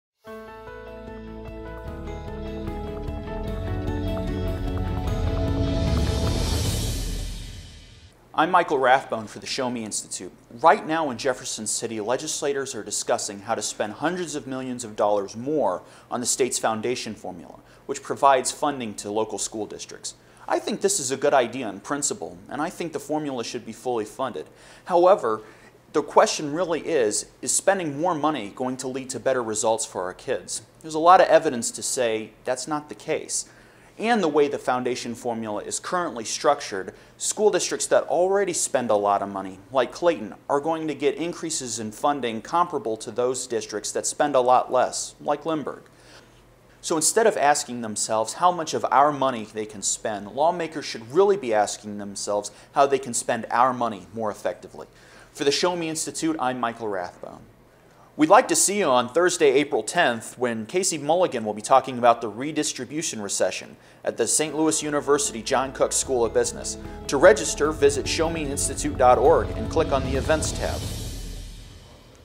Show-Me Now! Michael Rathbone on Education Funding
Legislators in Jefferson City want to increase education spending, but is that the best way to improve student outcomes in Missouri? Evidence suggests that increases in public school funding have little impact on student performance:

Register for our April 10th speaker series with Casey Mulligan on the Redistribution Recession: